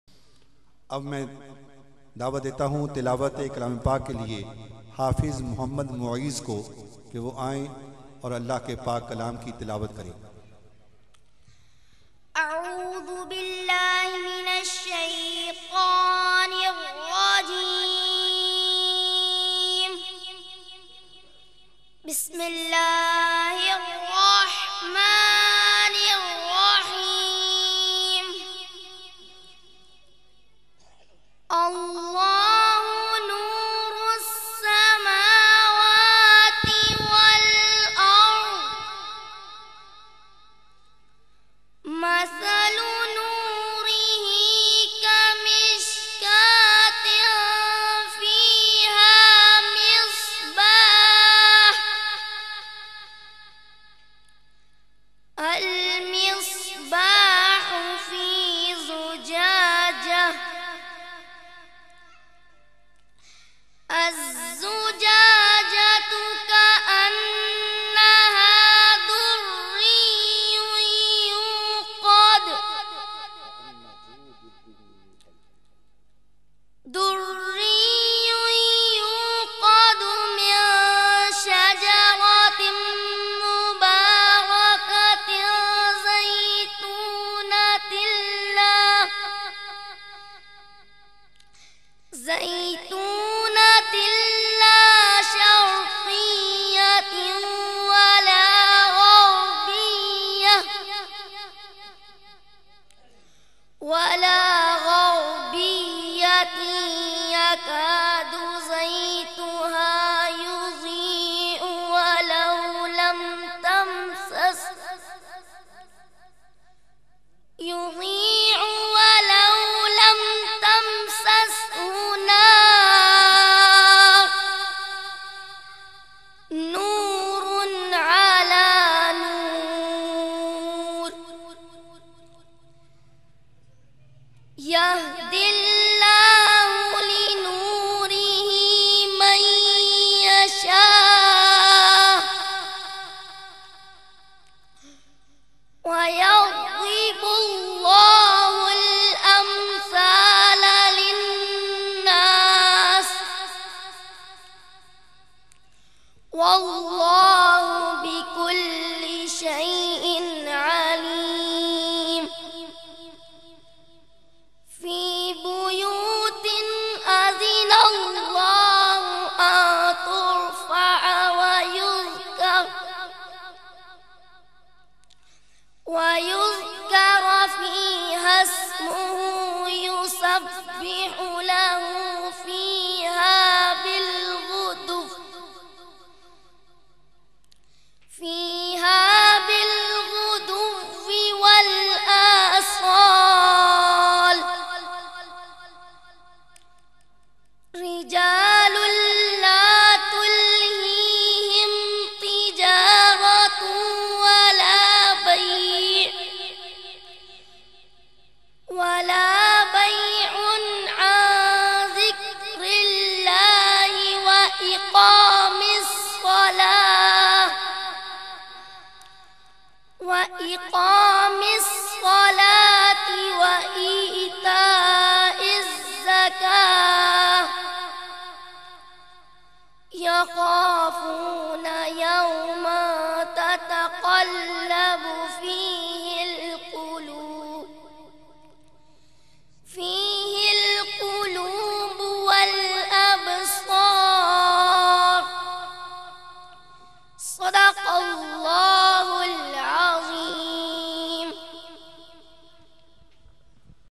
Tilawat by Hafiz Mueez
Hafiz Mueez in  Mehfil-e-Milad-e-Mustafa (Sallalaho Alai Wasalam) held at Malik Muhammad Javed home on February 25, 2011